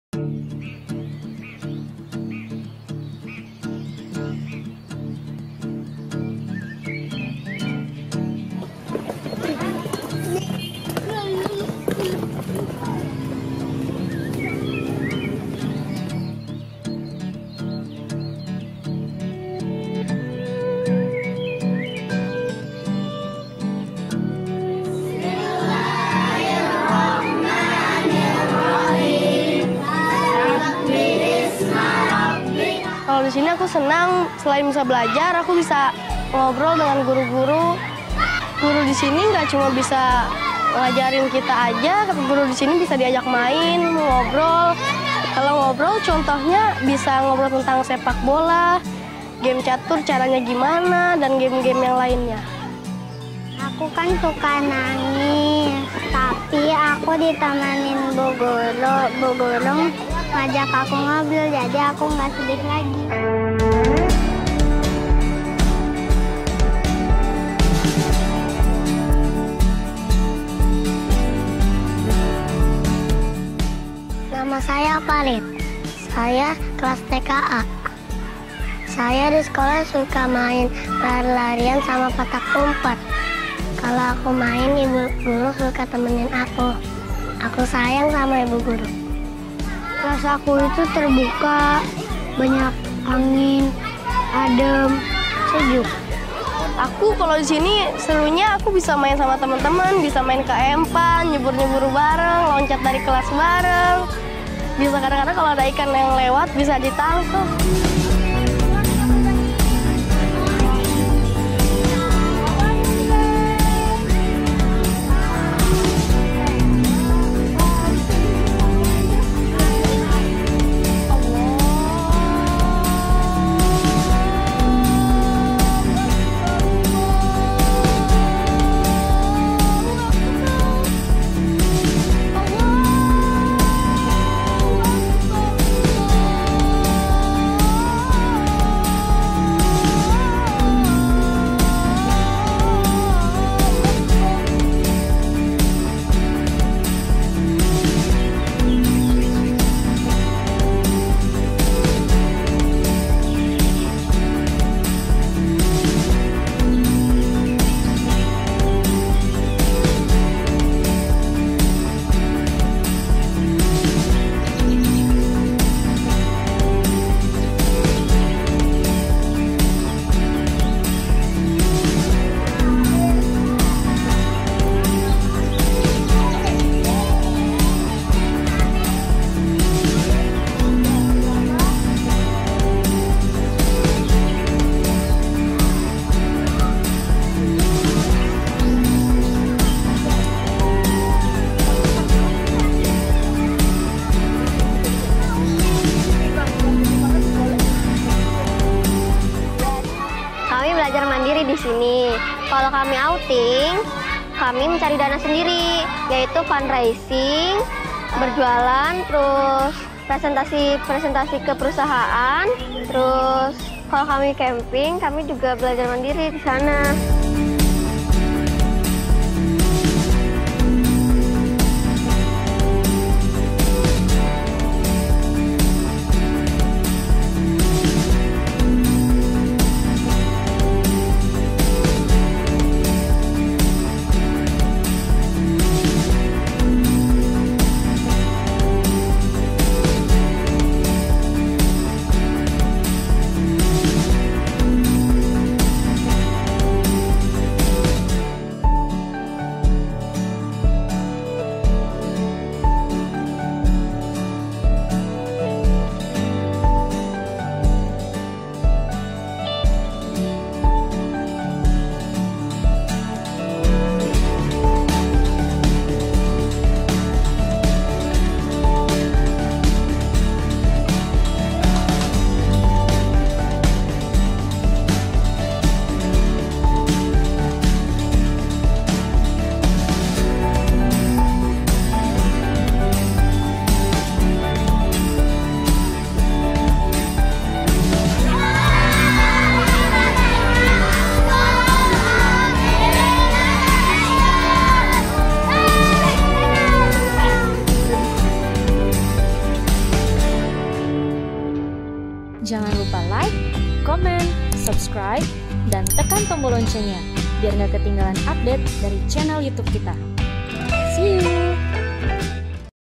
Sekolah Alam Indonesia - School Profile
Sekolah Alam Indonesia adalah halaman bermain mereka, sekaligus tempat belajar mereka. Anak anak dibebaskan menjadi diri mereka, mengembangkan potensi untuk tumbuh menjadi manusia mandiri, berkarakter, berakhlak mulia, berwawasan ilmu pengetahuan dan siap menjadi pemimpin khalifah fil ardh.